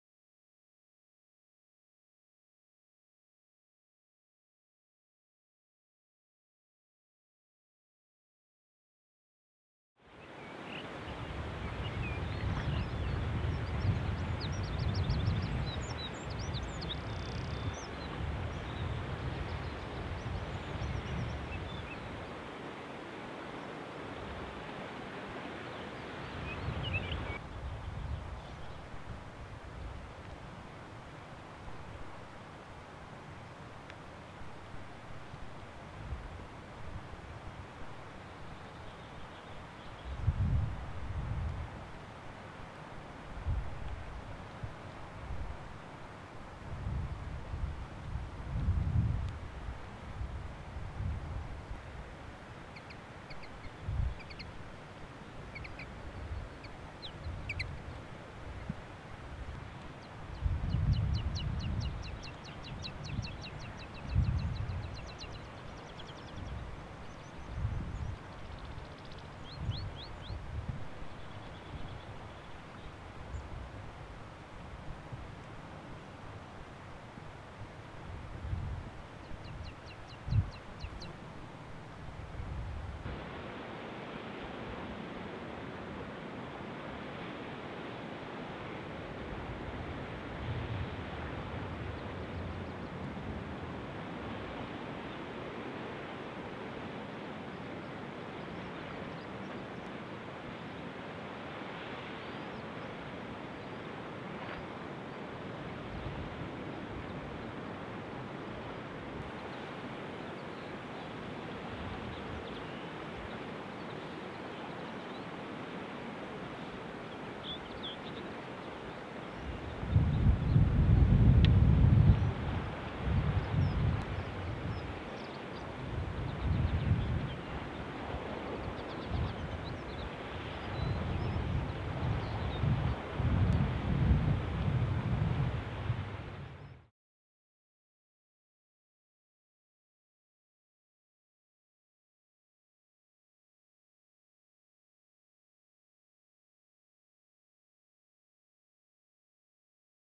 Lundy Island Special 7 - East Coast Path
A short visit as it was a very windy day (hence the shaky footage) to the East Coast path of Lundy, which has a very good walking trail passing by a few landmark locations of the island's past. But if it's wildlife you're after, you will need to be a little more patient as you must walk carefully and keep a lookout across the sea and on the paths. Saying that, I had some mixed spottings such as a Male Linnet, a herd of Sika Deer, a mini meadow of Bluebells and a Gannet out looking for fish.

Please also check my Facebook and Instagram pages for my photography, thank you!